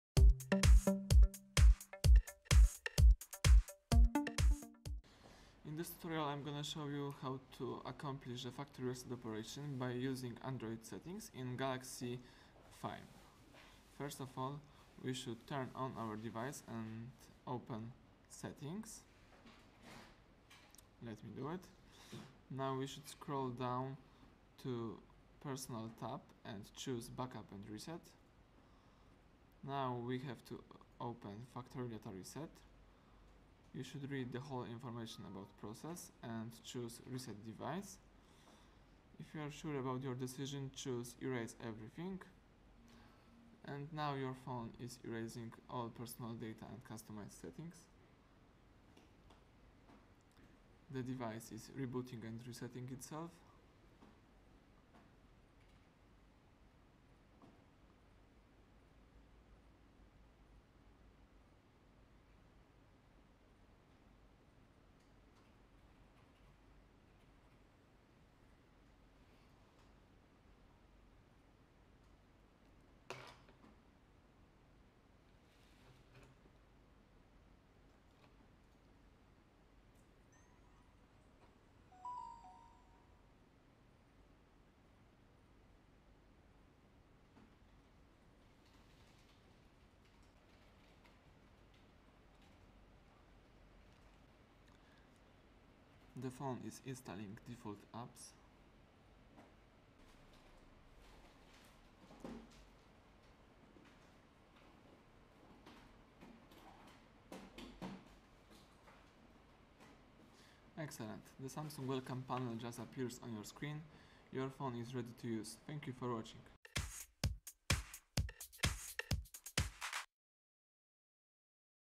Hard Reset SAMSUNG S6810 Galaxy Fame - factory reset tutorial
How to perform the hard reset? How to accomplish factory reset?
How to accomplish full reset? How to erase all data na settings?
A master reset restores the original factory settings and delete your personal data on the internal storage, such as downloads, ringtones, images, apps, contacts etc.